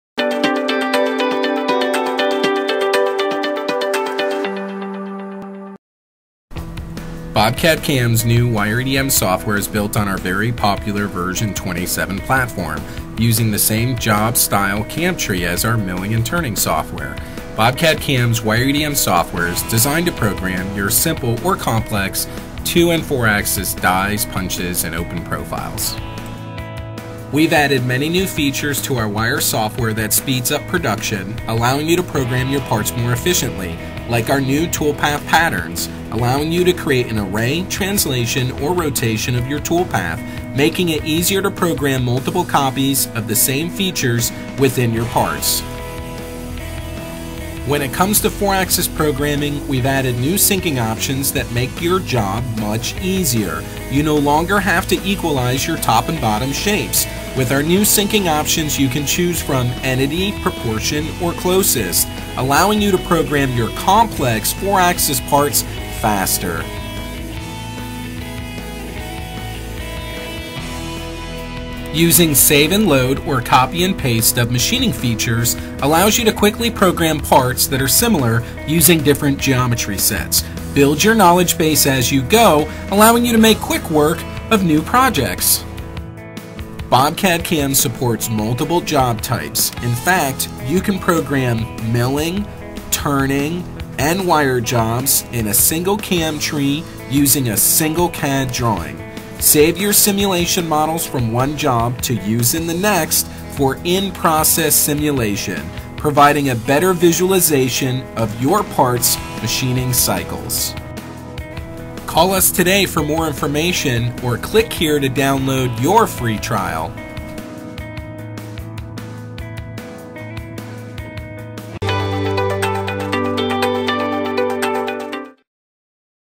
BobCAD-CAM V27 Wire EDM Software
BobCAD-CAM offers precision manufacturers its 2 & 4 Axis Wire EDM CAD-CAM software for programming simple or complex wire toolpaths and advanced simulation for Wire EDM part-making.

BobCAD-CAM Corporate
    28200 U.S. Highway 19 N.

International Offices
    Columbia Office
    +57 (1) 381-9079
    Mexico Office